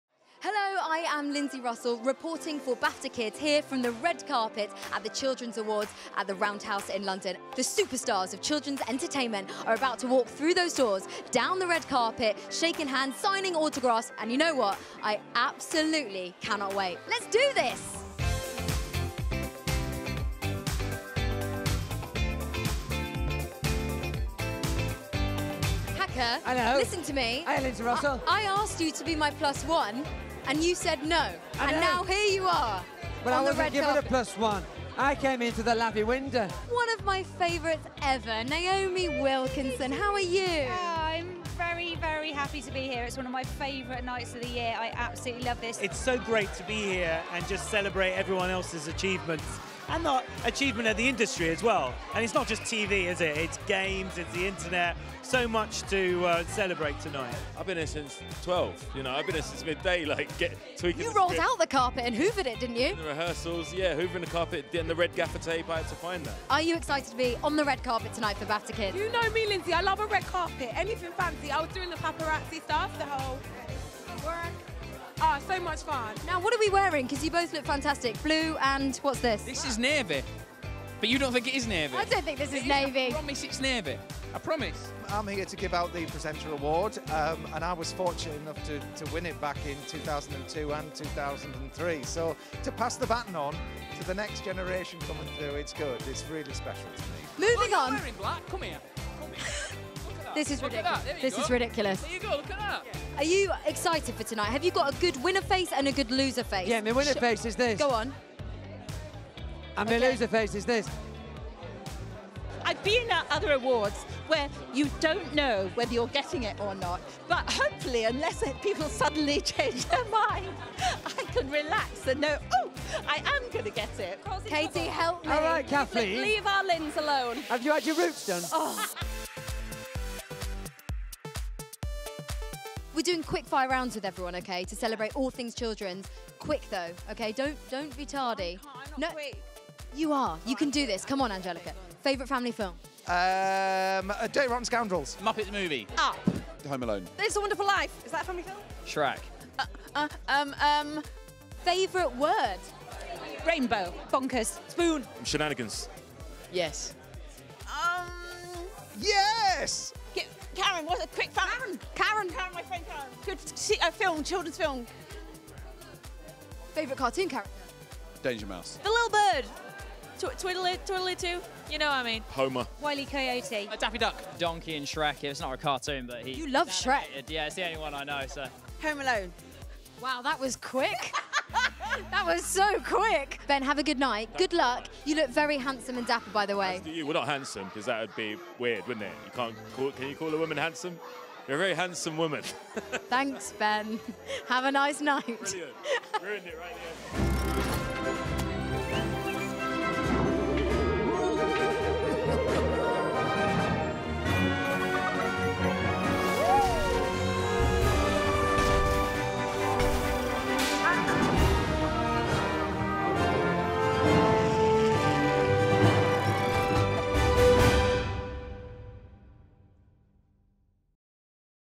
Lindsey Russell on the BAFTA Children’s Red Carpet | Highlights
Lindsey Russell chats with the superstars of kids’ television – see what The Vamps, Matt Baker and more have got to say on the red carpet.